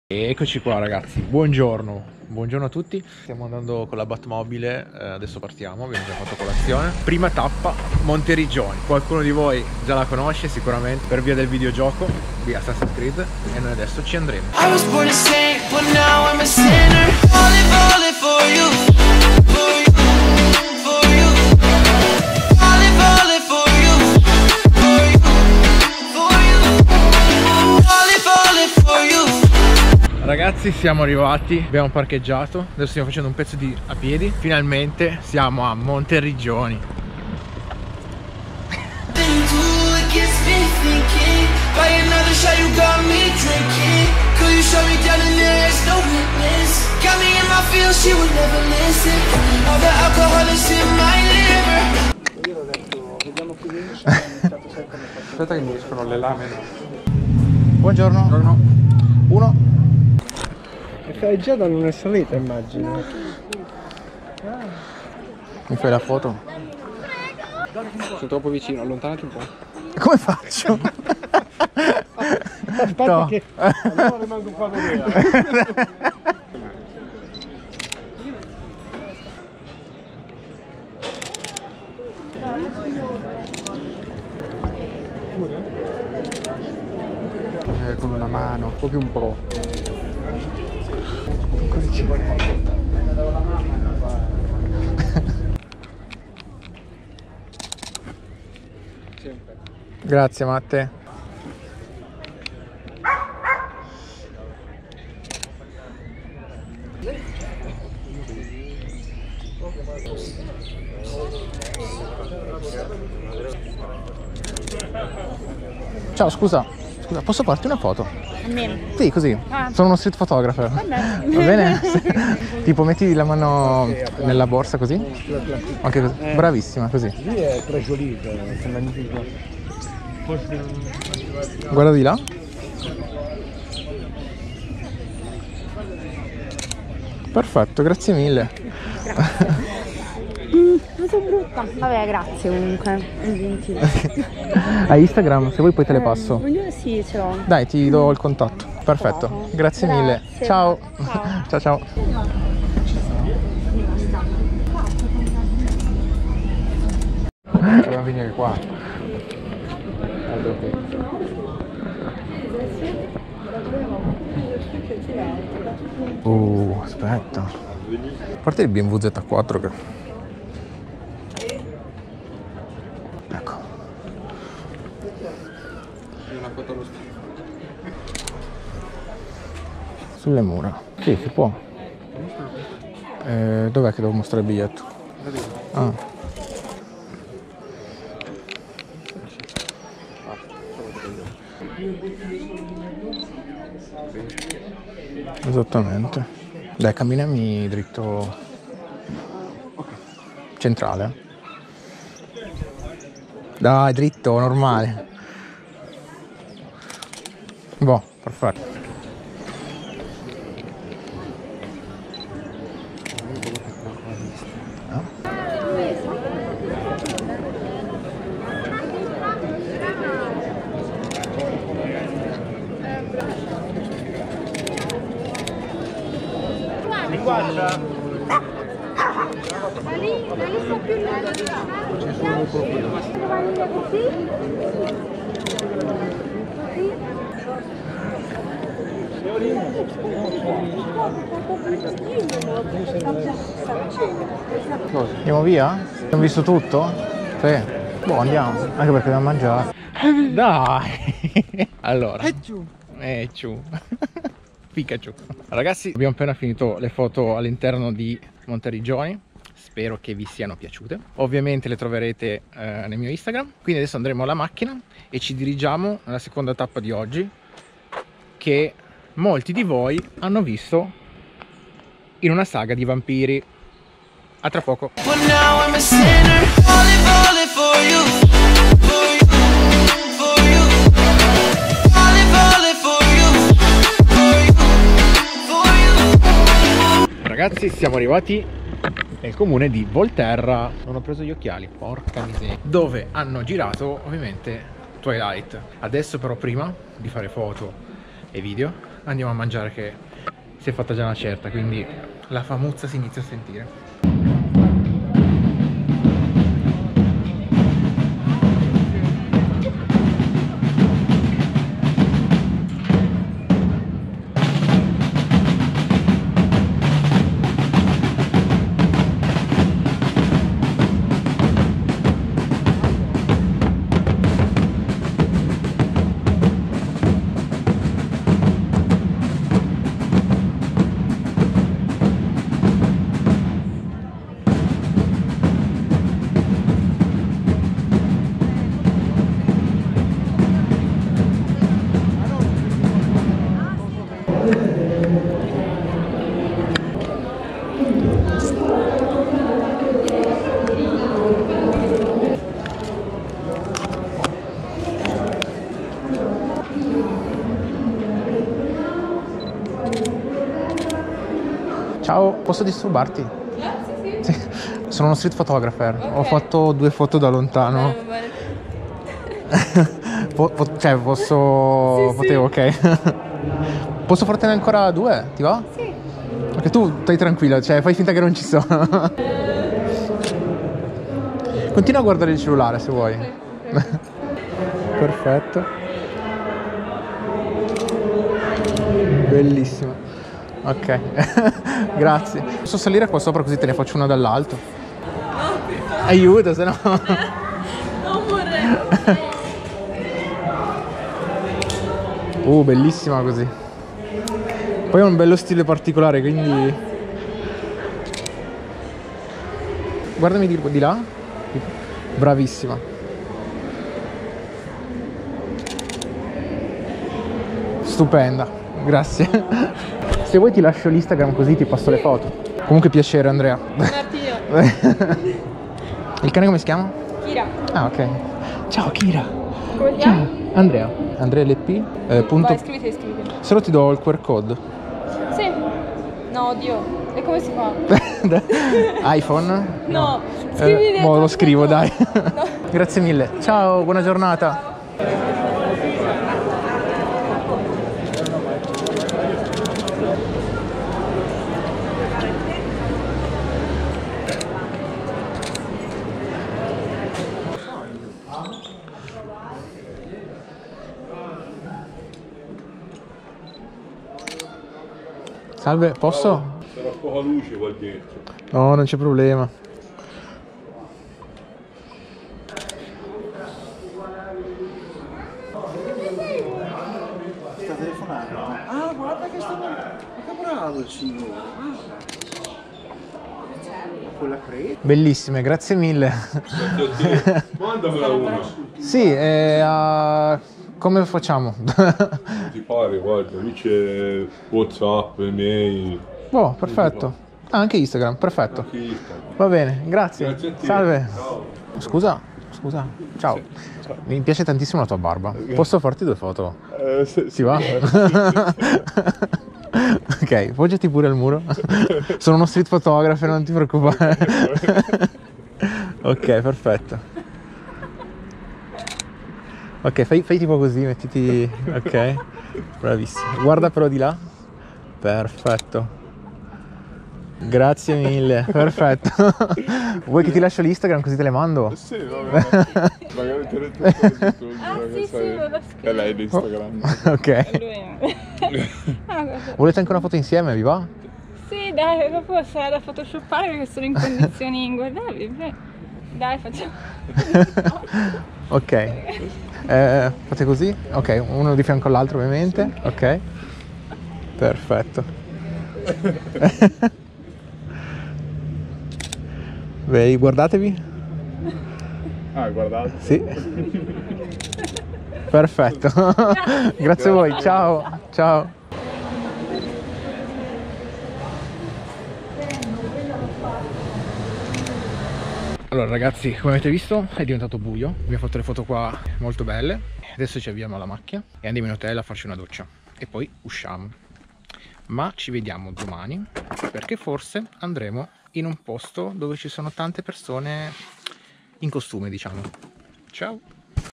Monteriggioni e Volterra "street photography"
Secondo giorno in toscana , che ci porta tra le mura di Monteriggioni (SI) famosa per il video game Assassin's creed e tra le vie di Volterra (PI) dove sono stare girate alcune riprese del film di Twilight .